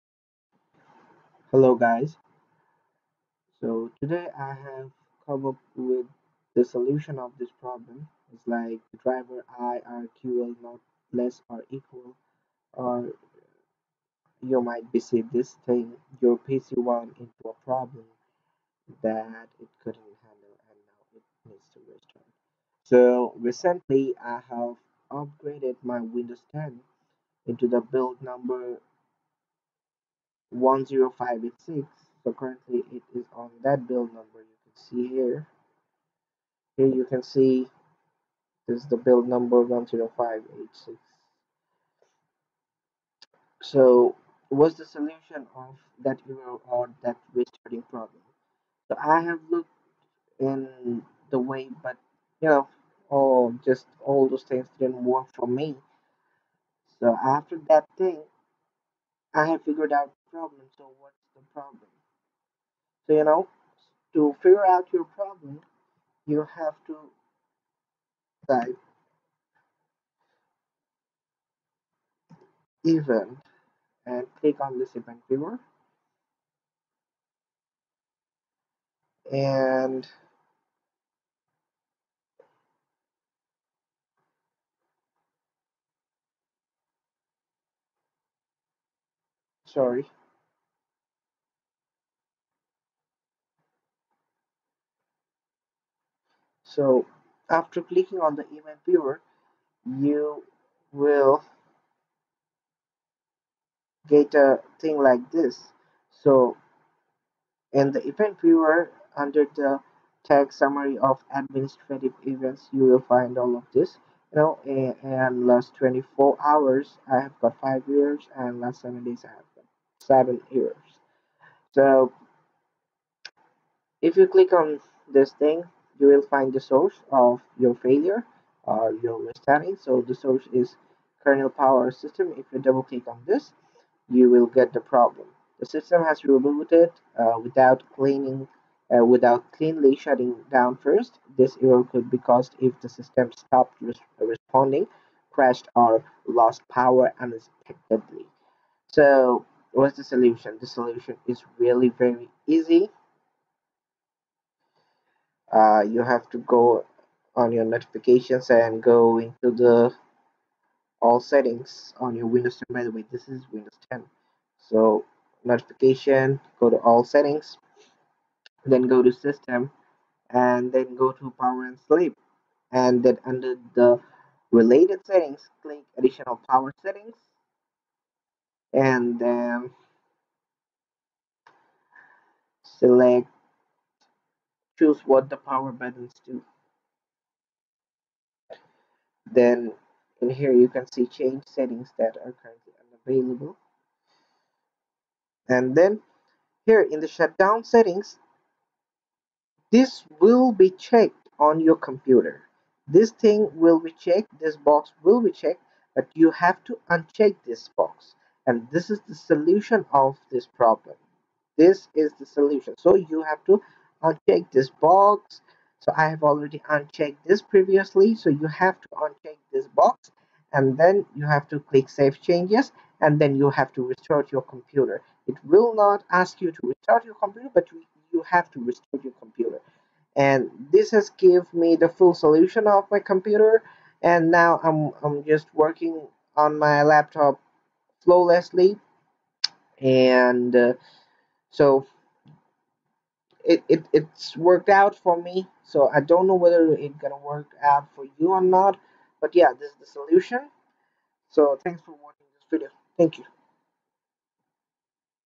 How to fix driver_IRQL_NOT_LESS_OR_EQUAL Error in windows 10/kernel-power
/The system has rebooted without cleanly shutting down first. This error could be caused if the system stopped responding, crashed, or lost power unexpectedly/ kernel-power